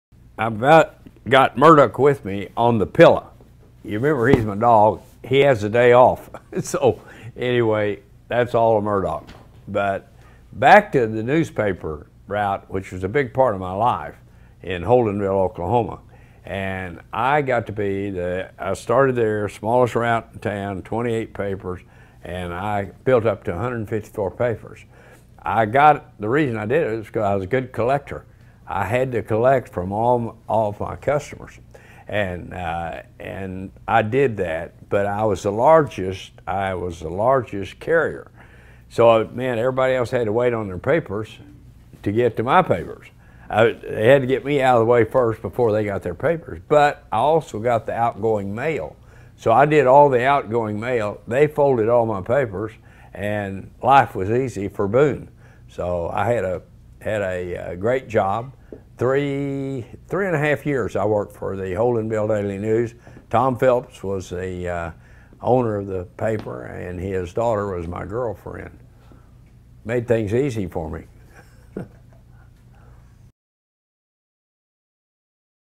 T. Boone Pickens Reminisces About Being A Paperboy
T. Boone Pickens, Chairman and CEO of BP Capital, recalls his early job as a paperboy in Holdenville, Oklahoma.